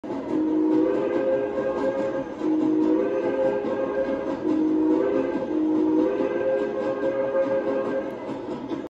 Check Out What I did!
I gave Prowler901’s C-41 Whistle Smoke!